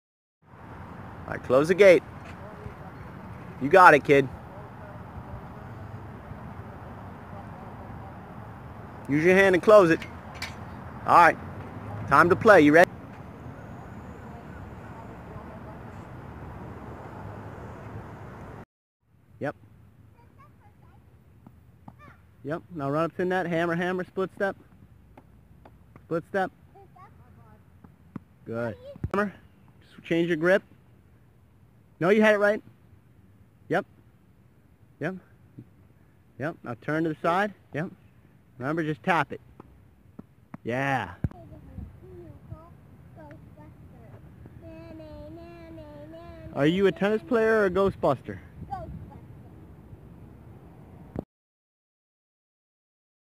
Toddler Tennis part 3 Feb to June 2019
In this video, grip change and learning volley
tennis hacks